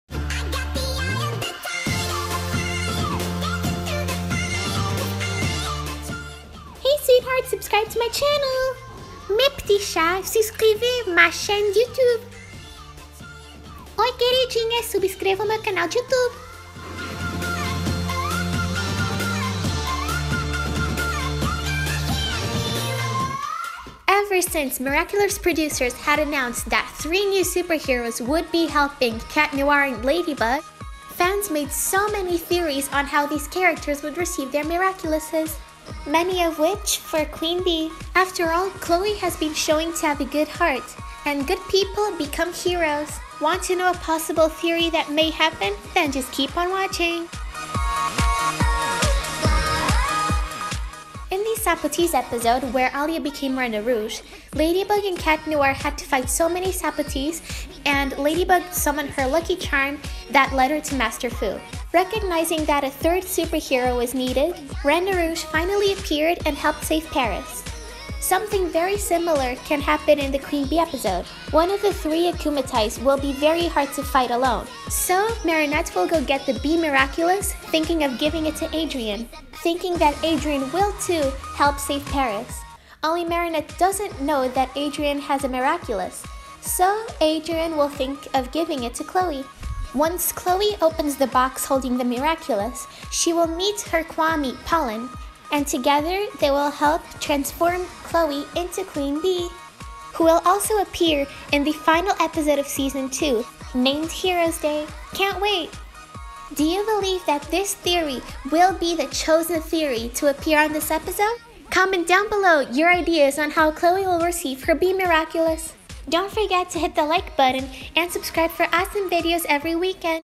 MIRACULOUS LADYBUG - HOW WILL QUEEN BEE RECEIVE HER MIRACULOUS?💖💖
💖💖QUEEN BEE IS SAID TO STAR ON THE “STYLE QUEEN” EPISODE, ON THE 29TH OF JUNE, BUT WE JUST COULDN’T WAIT TO SEE HOW CHLOÉ GOT HER MIRACULOUS!! THAT’S WHY I MADE A VIDEO ON THE MOST PROBABLE THEORY!! DO YOU BELIEVE THAT THIS THEORY WILL HAPPEN? OR DO YOU BELIEVE IN A DIFFERENT THEORY?